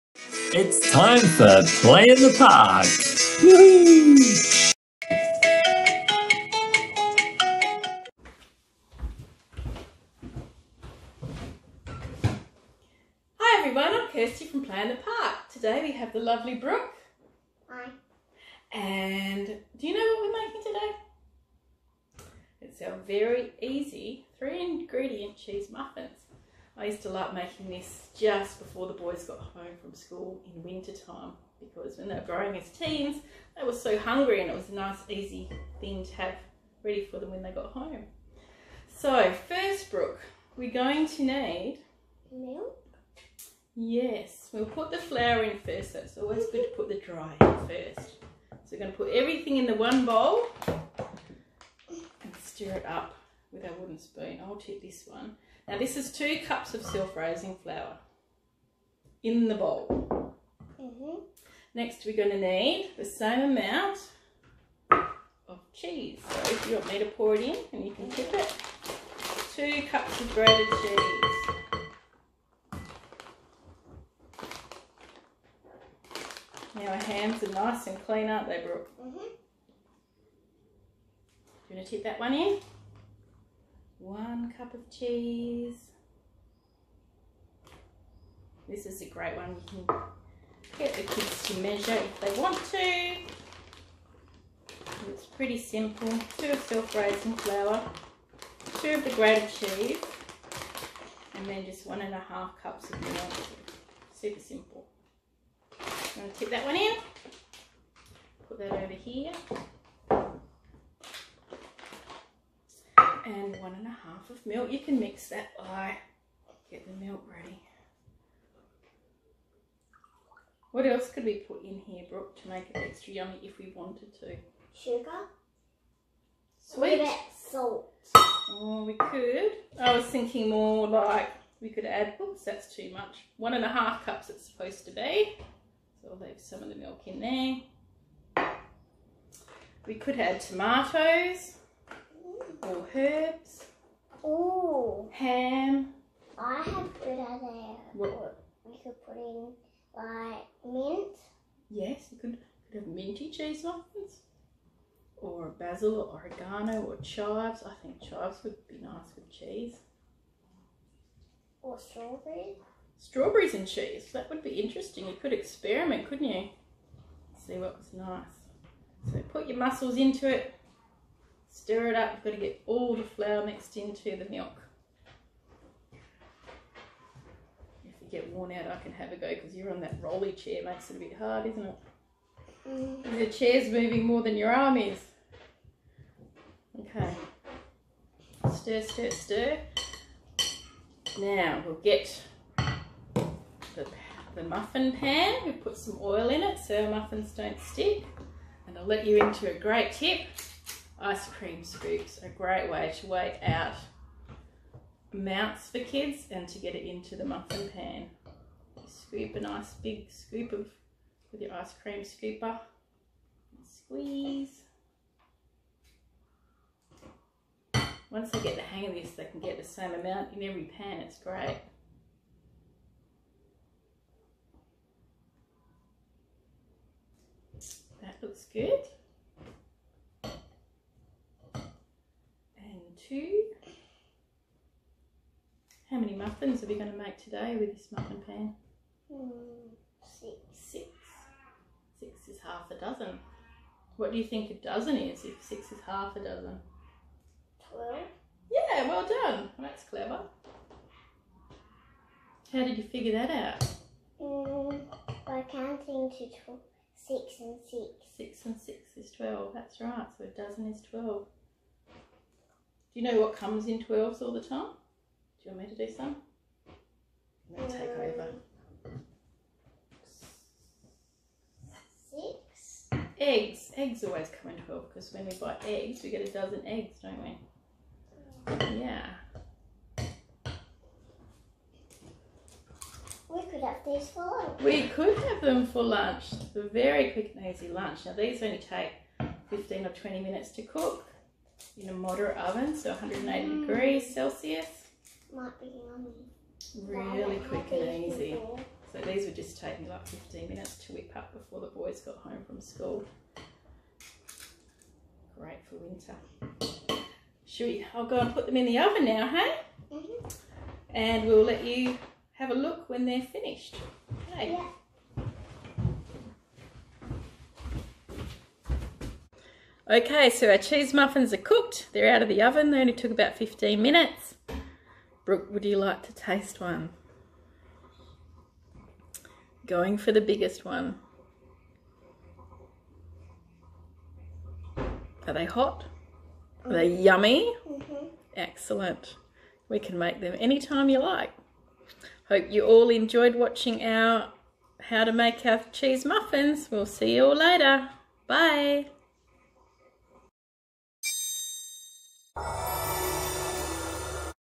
PIP Cooking Time # Cheese Muffins
YMCA WA Play in the Park online, Cooking with kids can be fun, especially with this easy cheesy muffins recipe with Kirsty and Brooke.